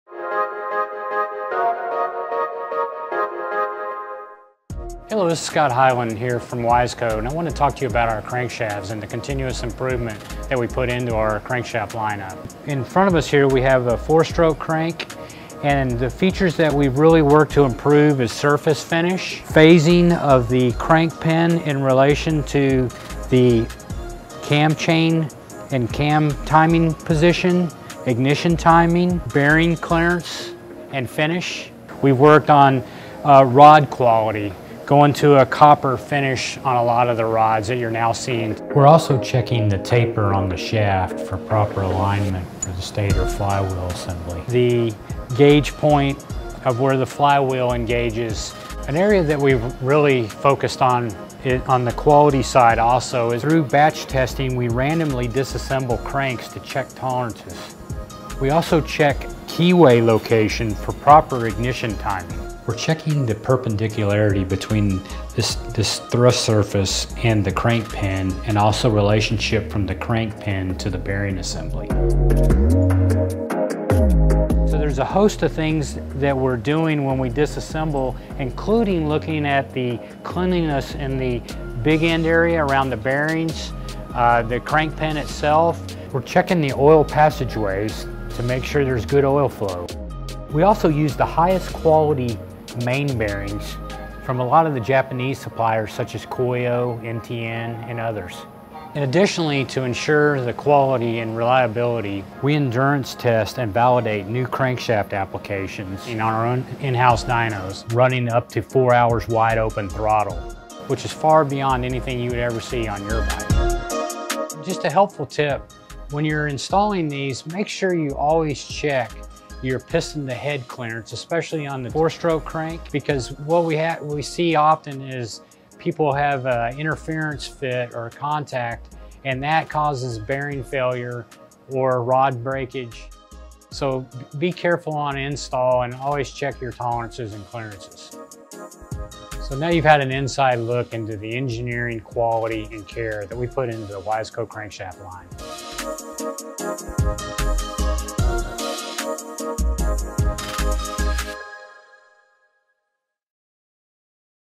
Wiseco Crankshafts Improved: Materials, Finishes & Quality Control
Year-in and year-out, we work to research and improve our entire line of performance products. Years of engineering and development have gone into making thorough improvements to our lineup of Wiseco crankshafts. Take a first-hand look directly into our engineering and quality control lab.